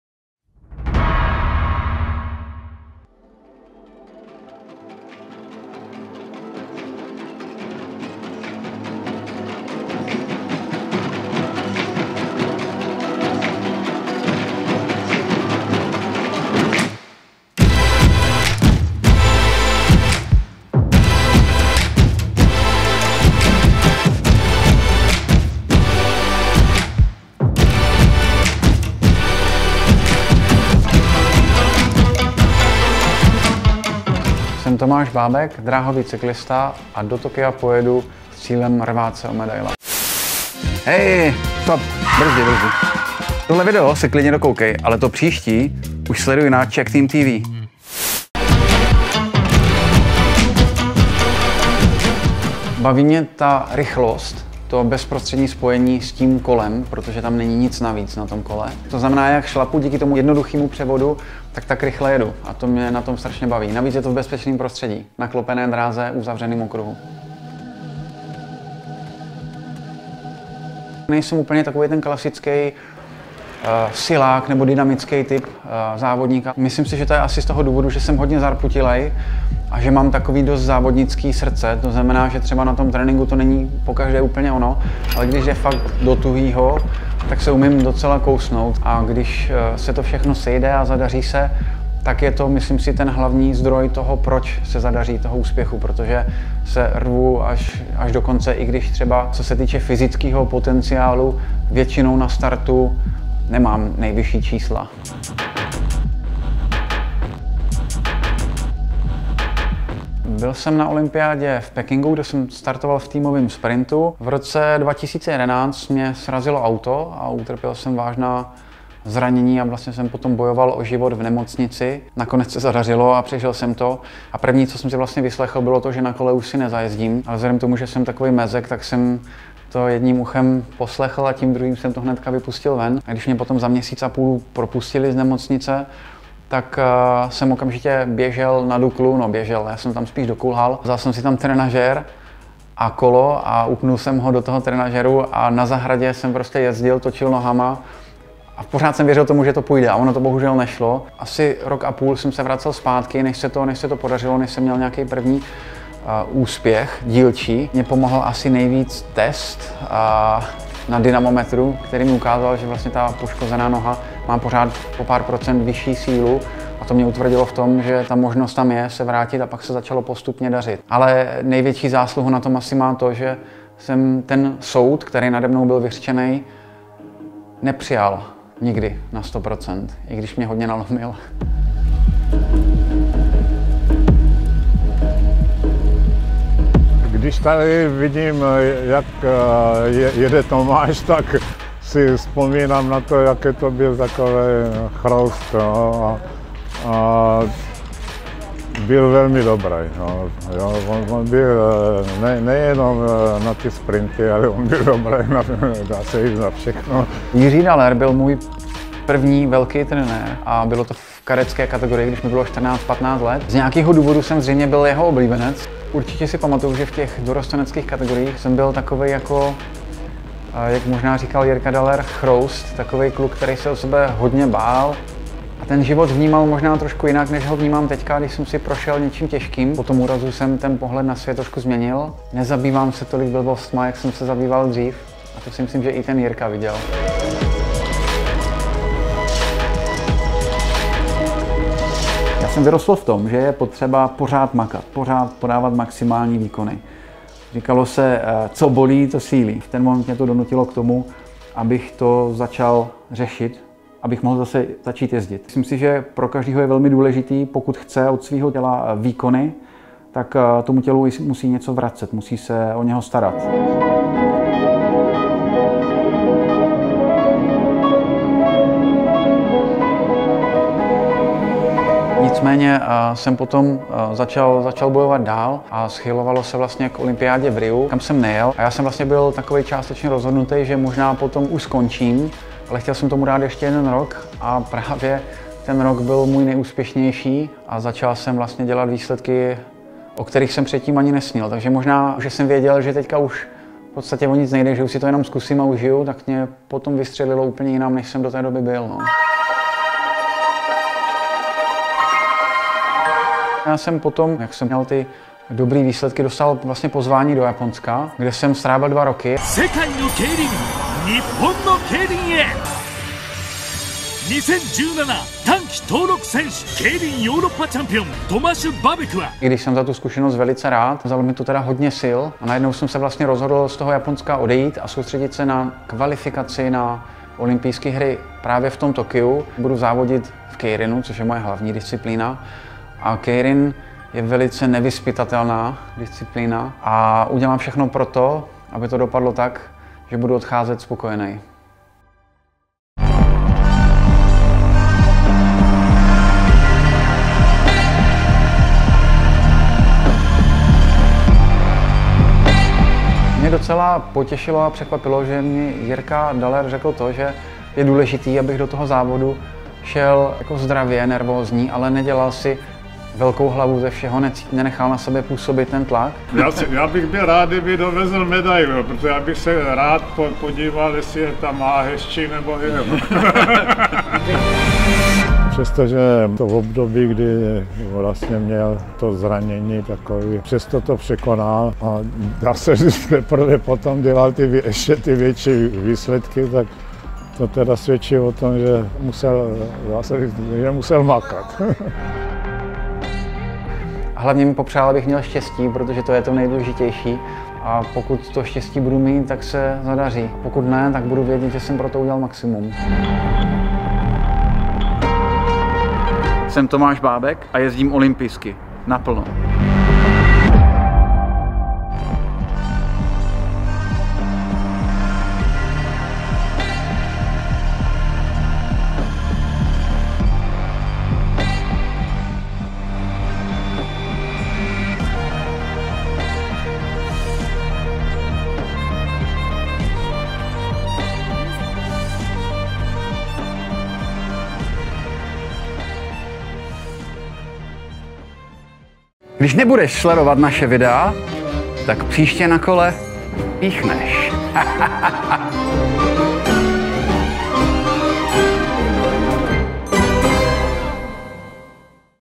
Do boje o medaile dám všechno | TOMÁŠ BÁBEK
Tomáš Bábek je český dráhový cyklista, který se jede do Tokia porvat o medaile. I přes vážnou nehodu se mu podařilo vrátit do sedla. Jeho vůle ho dostala na závodiště keirinu v Japonsku. Nyní je zpět v Japonsku a bude to opět velká jízda.
Buď s námi a ciť se olympijsky 🇨🇿.